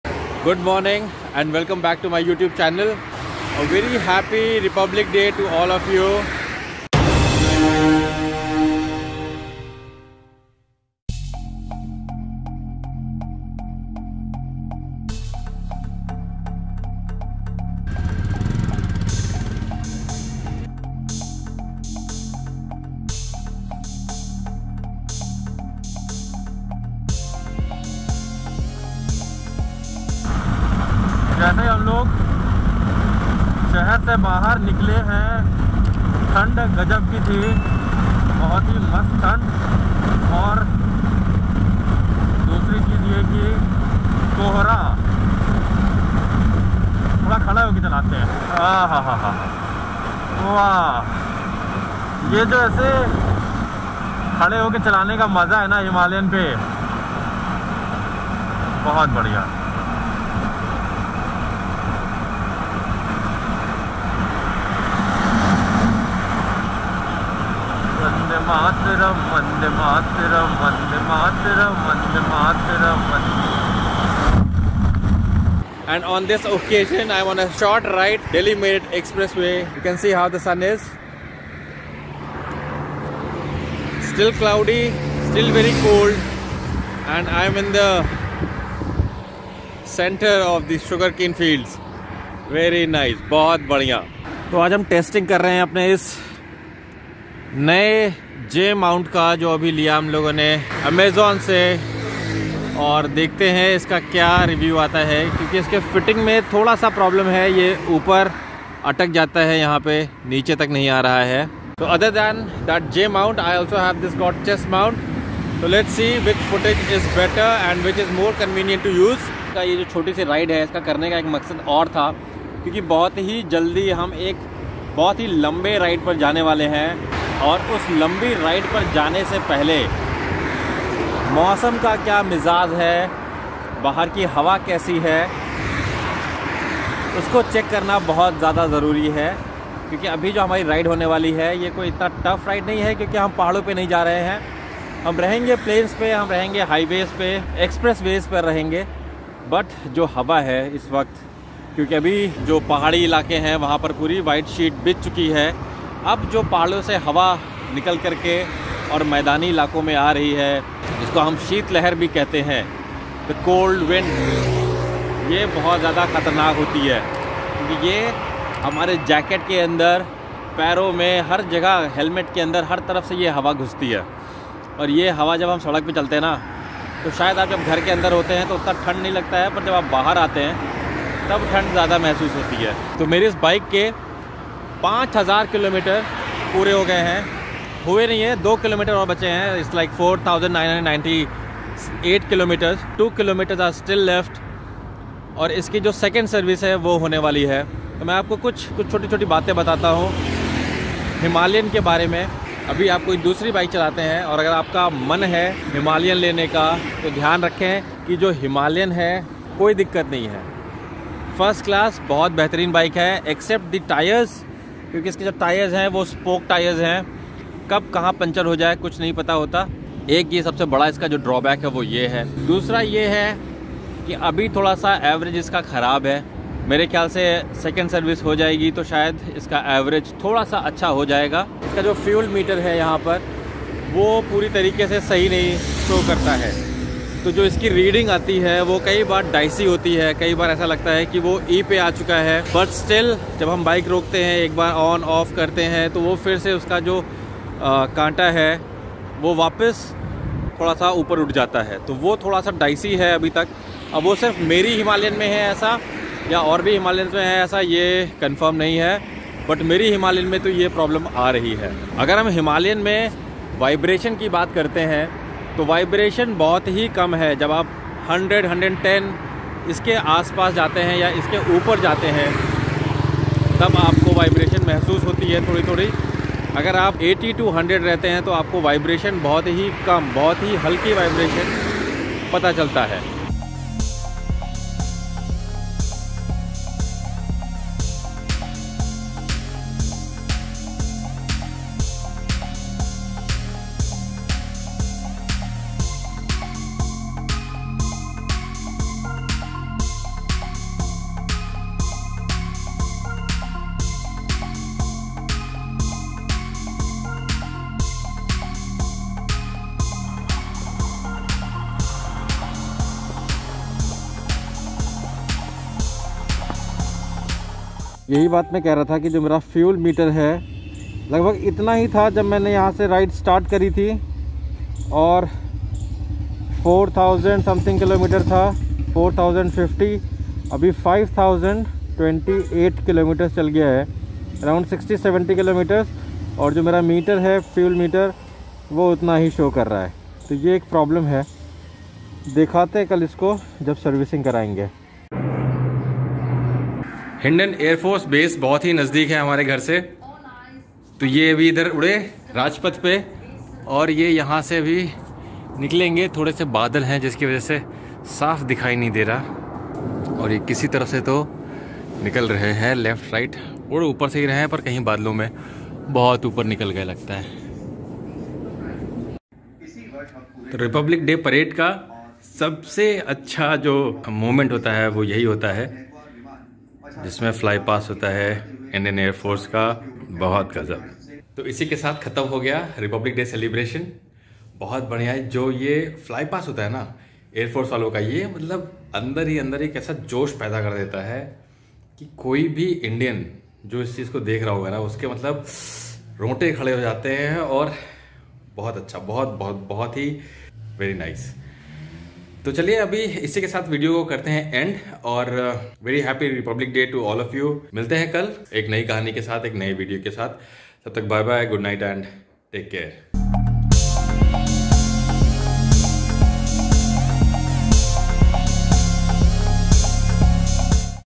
Republic Day Himalayan Ride | Dilli ki Sardi | Himalayan 5000kms review
First 5000 kms. have been completed on my Royal Enfield Himalayan. It has been a very wonderful riding experience, and not a single day on Himalayan was tiring.

As nothing is 100% perfect same goes with this motorcycle too, but surely i can say Himalayan is near to perfect machine in this price segment.